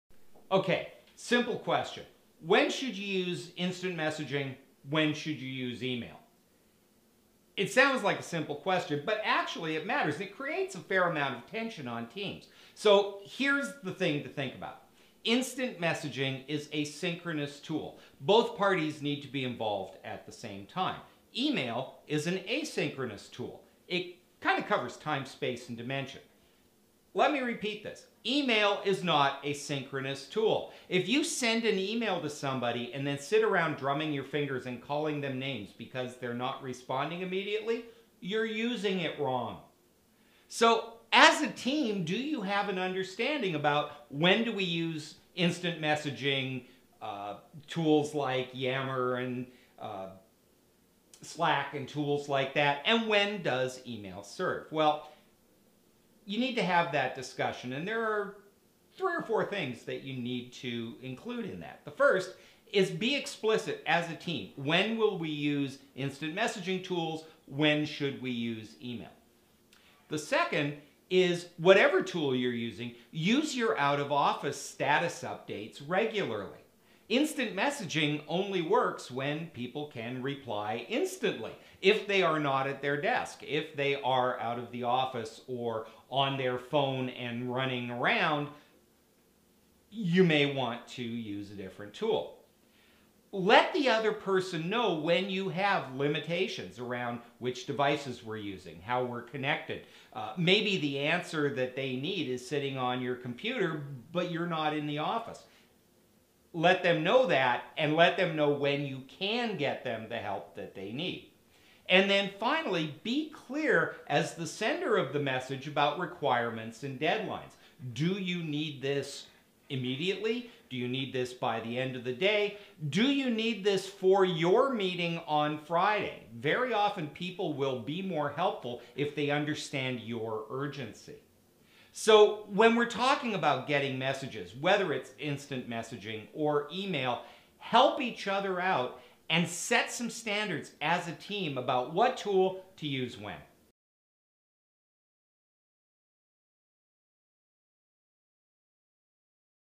Using IM vs. Email
A common problem on remote teams is not knowing when you should be using an instant messaging service like Slack or when you should communicate via email. Find out how you should handle this within your remote team.